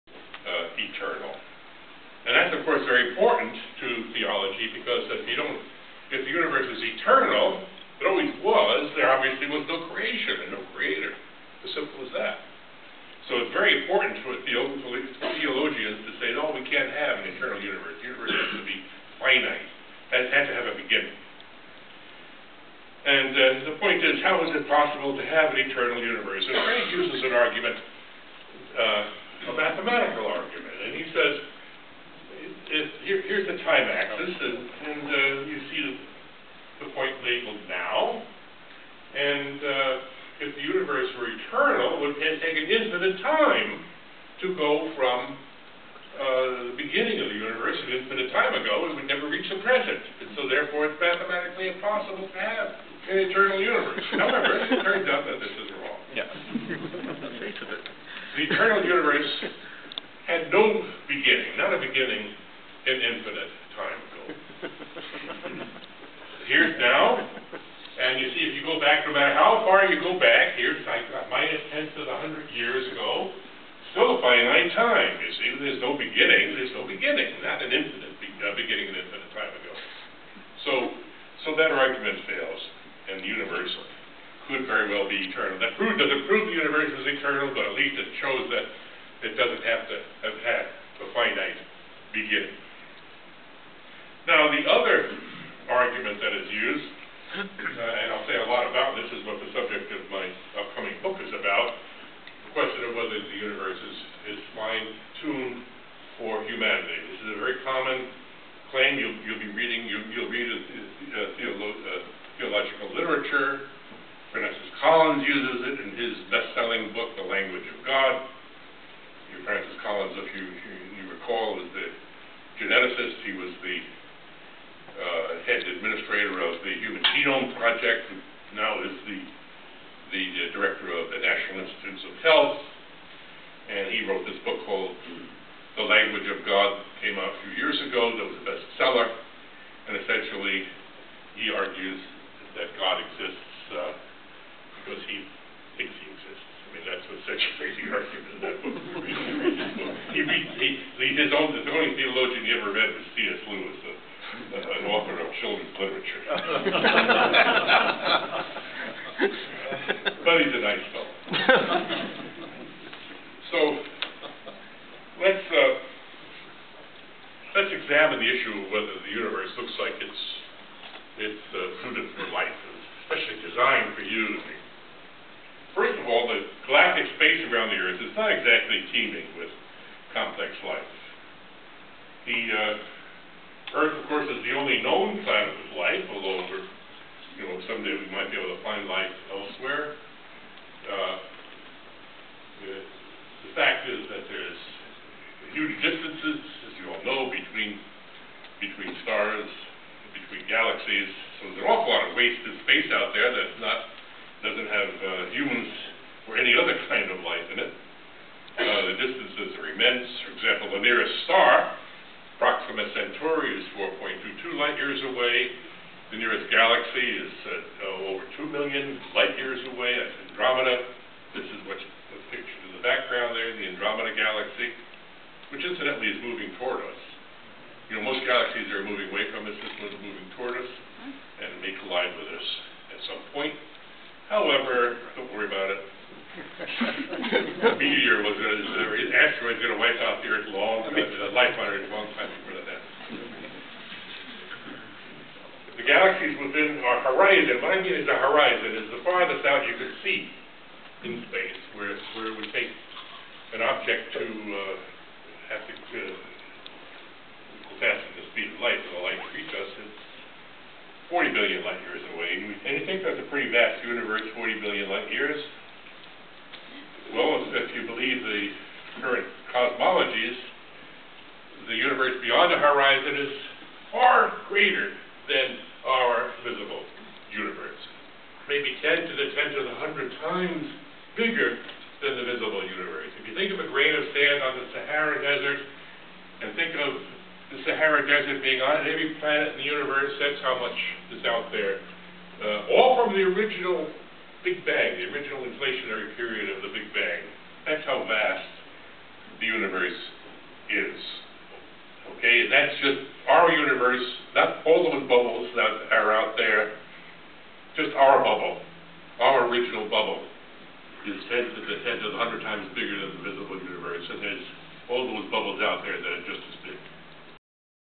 Victor J. Stenger 2: THE UNIVERSE IS LIKELY ETERNAL
A The Universe does not have to have a finite beginning.
Is the Universe fine-tuned for humanity, made for us in finite time?  See Stenger's upcoming book, "The Fallacy of Fine-Tuning."
The Universe is not teeming with complex life.  There are huge distances between galaxies; wasted space out there.  Andromeda galaxy is moving toward us but don't worry about that--an asteroid will wipe us out long before that.
The Universe beyond the horizon is FAR LARGER than our visible Universe of 40 billion light-years.  Our Universe, our bubble, is vast.